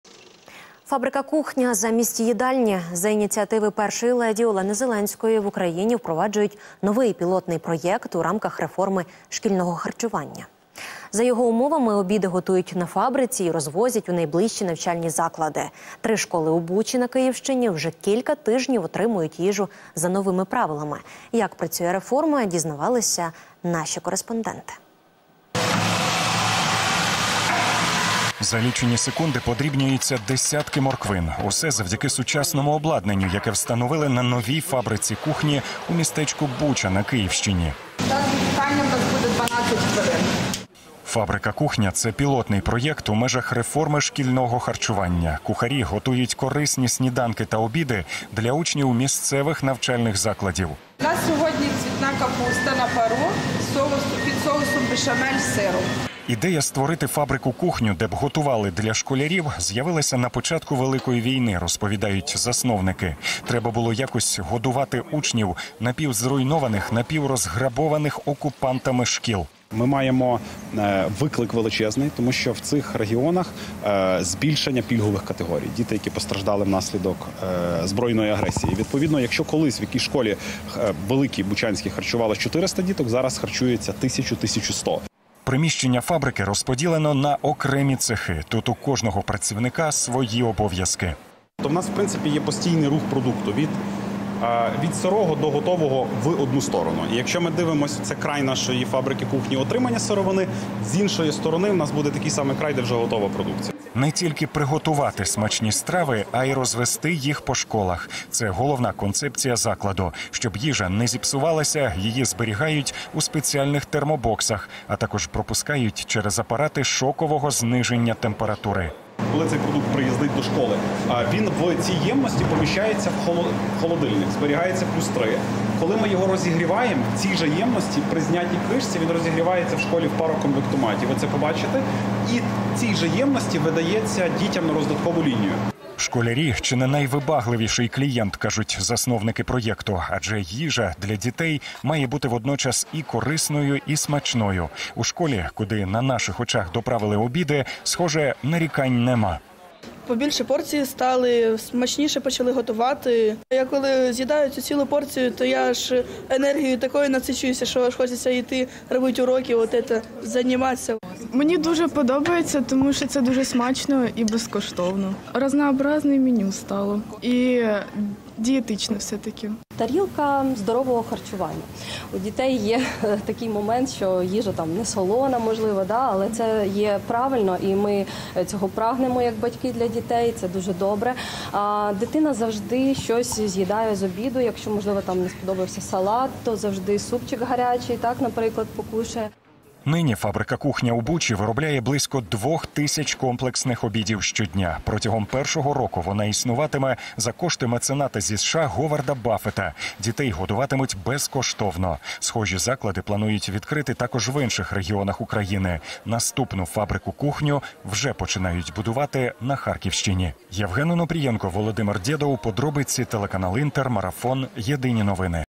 Фабрика-кухня в Бучі: як працює пілотний проєкт у межах реформи шкільного харчування
Фабрика-кухня замість їдальні. За ініціативи першої леді Олени Зеленської, в Україні впроваджують новий пілотний проєкт у рамках реформи шкільного харчування. За його умовами, обіди готують на фабриці і розвозять у найближчі навчальні заклади. Три школи у Бучі на Київщині вже кілька тижнів отримують їжу за новими правилами. Як працює реформа - дізнавалися наші кореспонденти.